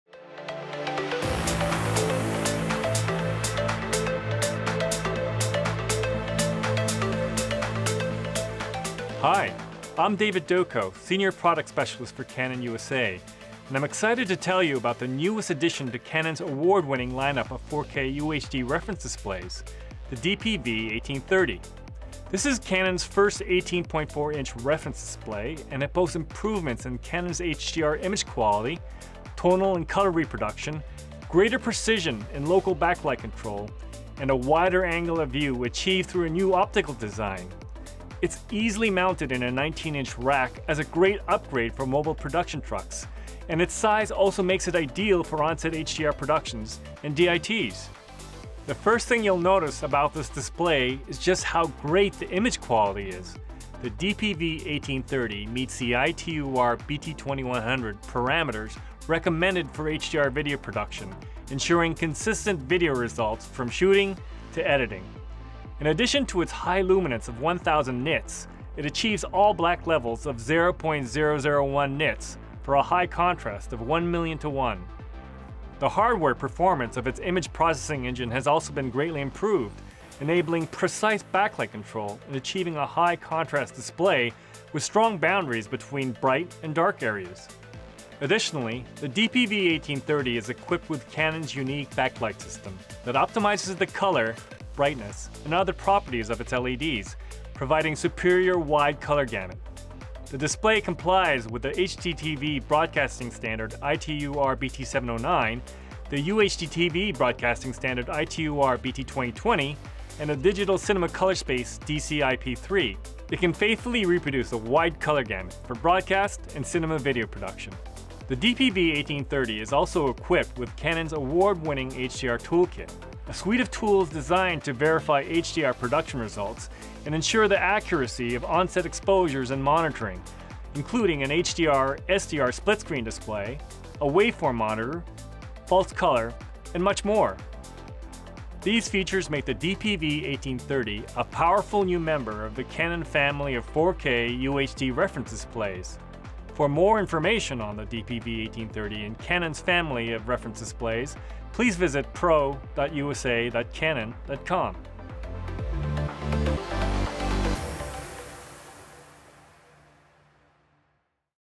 Introducing Canon's DP-V1830 4K Reference Display
At 18.4 inches, the new DP-V1830 4K Reference Display from Canon is rack-ready for mobile production trucks, and features a powerful suite of tools that make it an ideal solution for HDR or SDR productions.